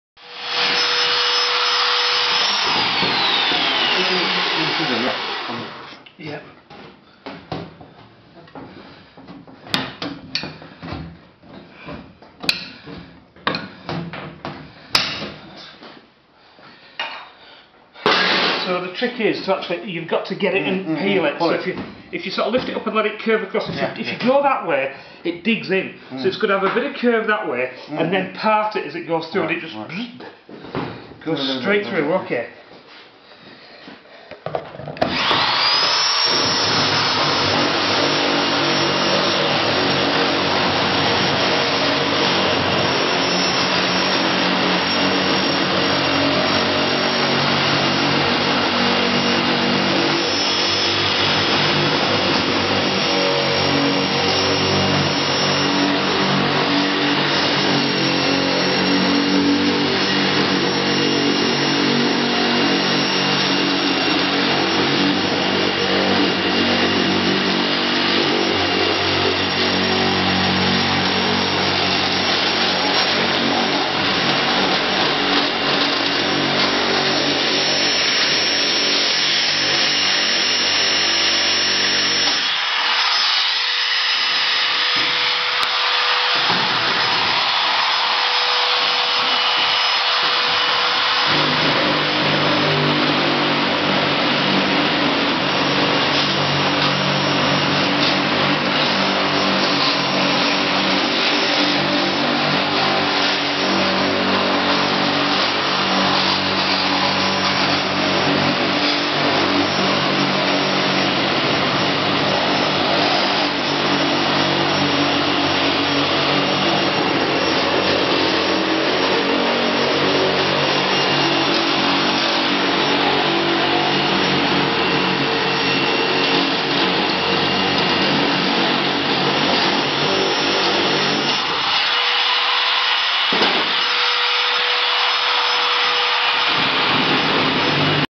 Chop Shop Leepu learning to use the English Wheeling machine at Andy Kissack's 4.AVI
Andy and Leepu cut out the Aluminium panels before Wheeling.